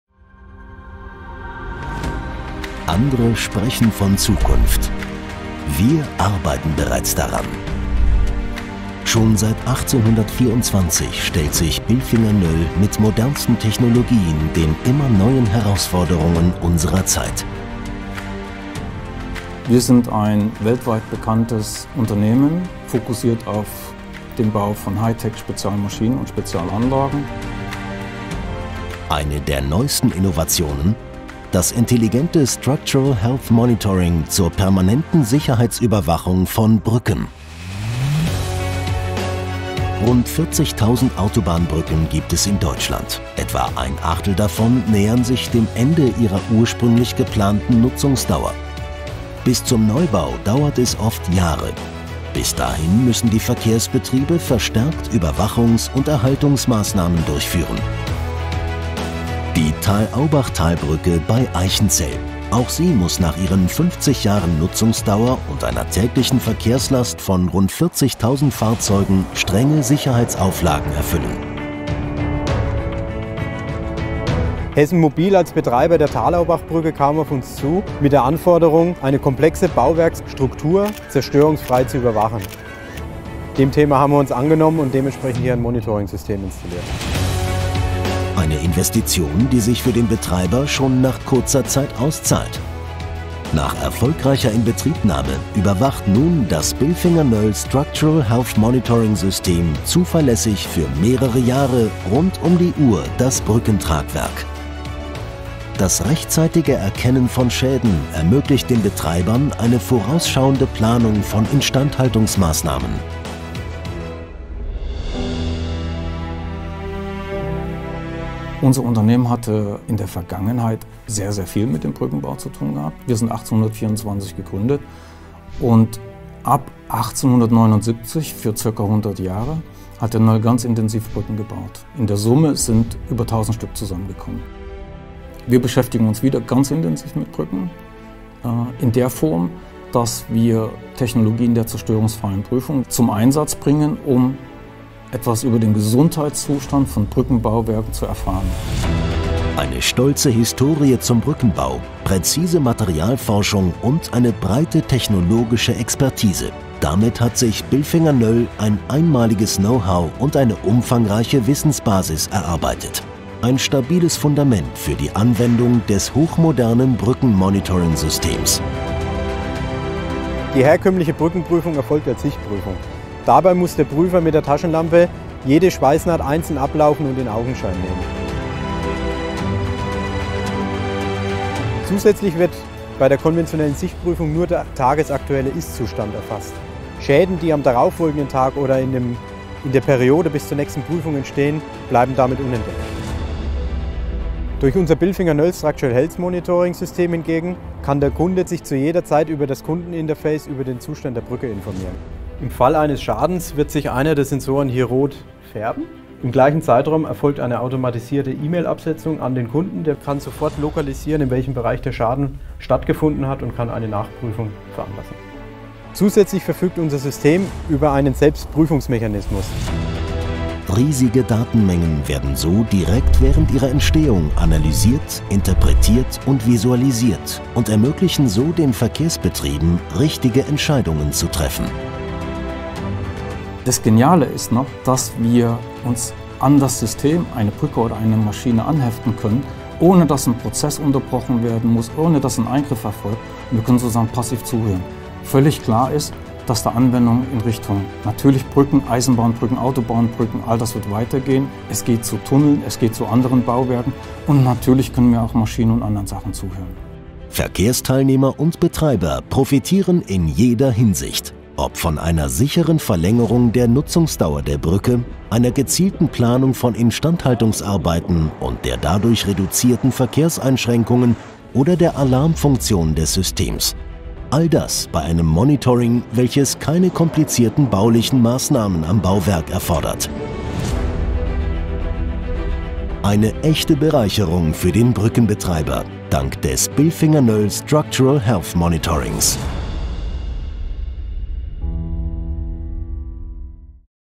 Bilfinger Dauer-Monitoring von Brücken
Mit innovativen Monitoring Methoden Risiken managen und Instandhaltungsaufwand reduzieren.
Das Bilfinger Dauer-Monitoring System der Bilfinger Noell ermöglicht eine ganzheitliche Dauerüberwachung gefährdeter Baustrukturen in Verbindung mit hochmoderner Informationstechnologie.